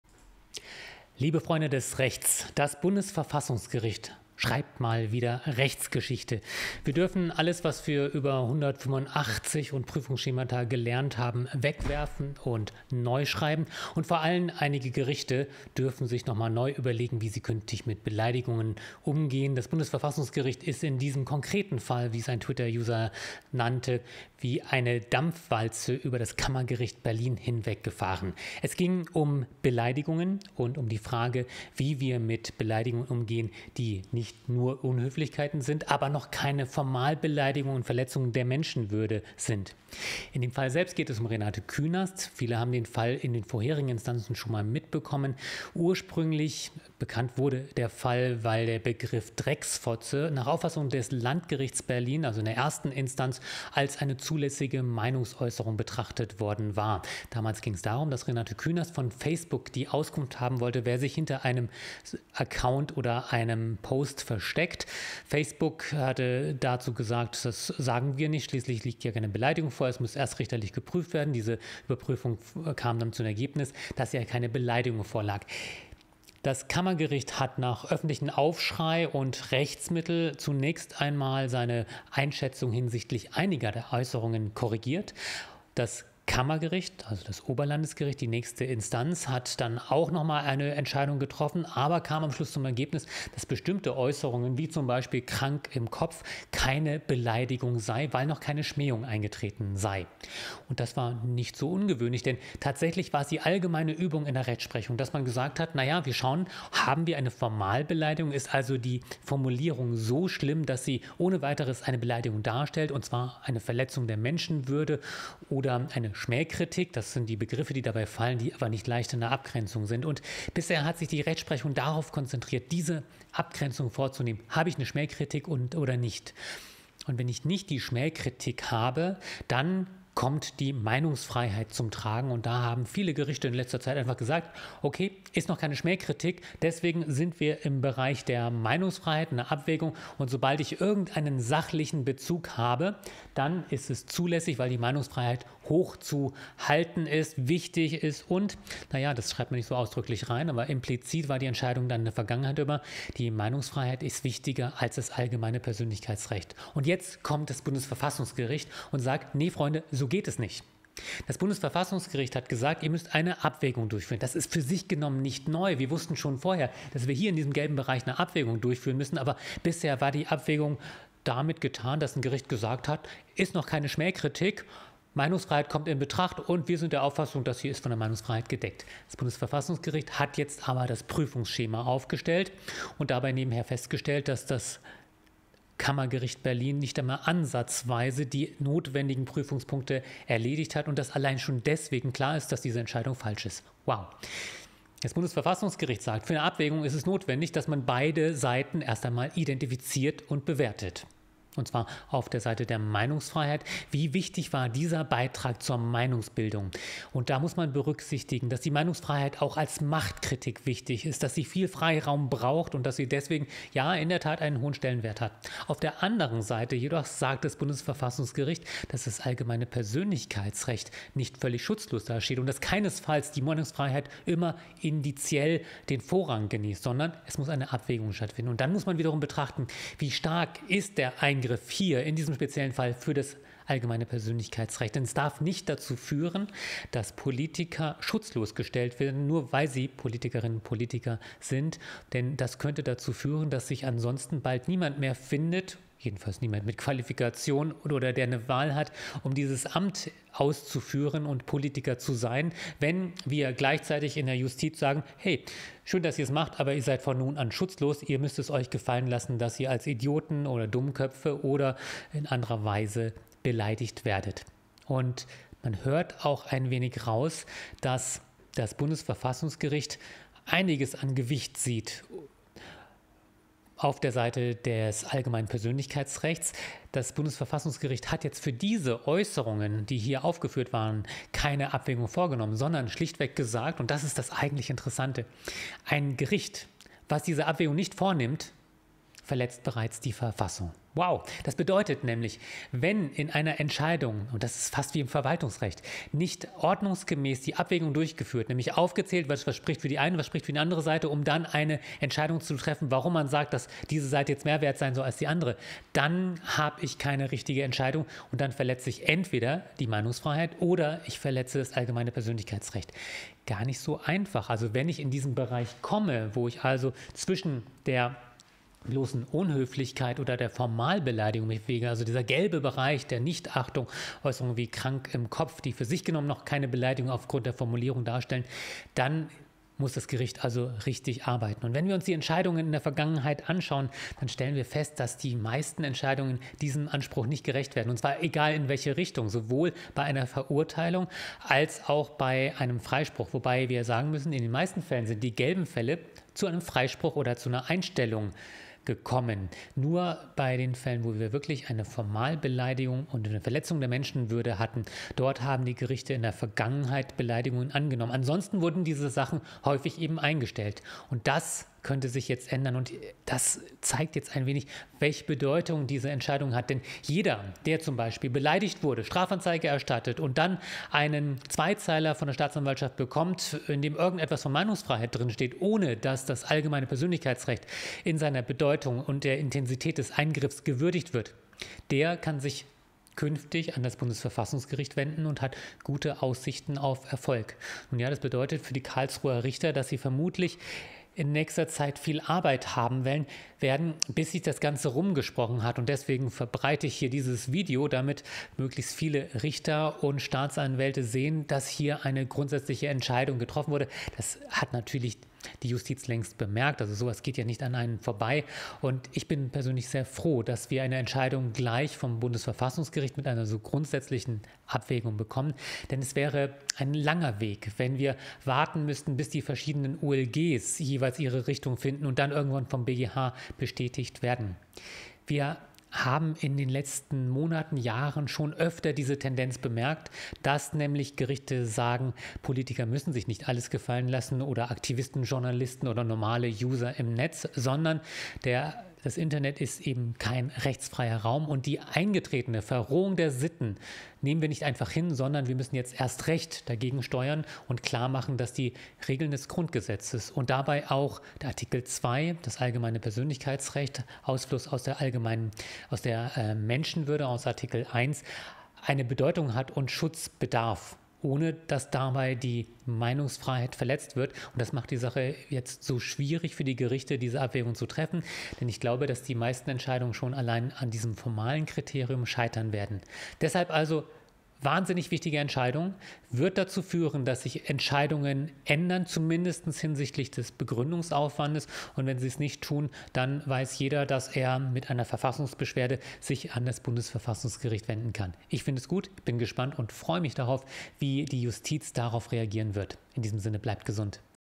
BVerfG kanzelt Berliner Richter wie Schuljungen ab und erfindet neue Spielregeln für Beleidigungen
Hinweis: Wir haben Fr. Künast bei diesem Verfahren nicht vertreten, sondern die Media Kanzlei.

Erstes Video mit dem neuen Tamron 35-150 f2.0/2.8 @80mm